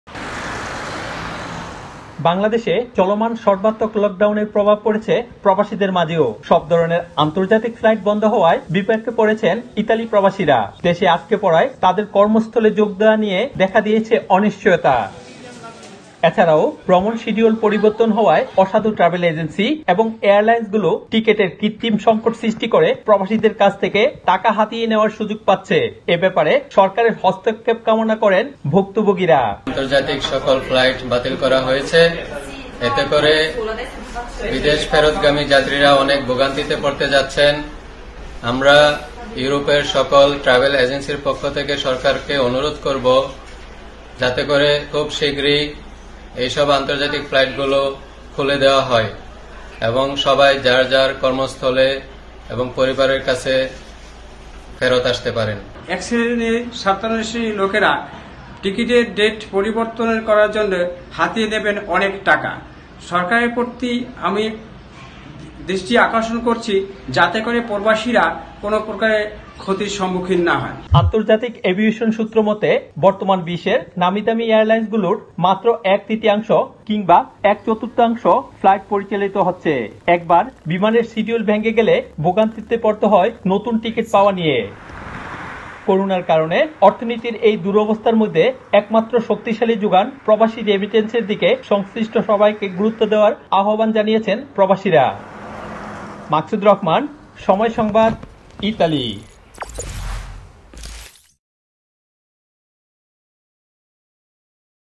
বাংলাদেশি লকডাউনে বিপাকে ইতালি প্রবাসীরা! | Italy Bangladesh Flight | International News
বাংলাদেশ সরকার ঘোষিত সর্বাত্মক লকডাউনে আন্তর্জাতিক বিমান চলাচল বন্ধ হওয়ায় বিপাকে পড়েছেন ইতালিসহ ইউরোপের প্রবাসীরা। দেশে আটকেপড়াদের কর্মস্থলে যোগ দেয়া নিয়ে অনিশ্চয়তার কারণে দুশ্চিন্তায় রয়েছেন তারা। ইতালি থেকে মাকসুদ রহমানের রিপোর্ট।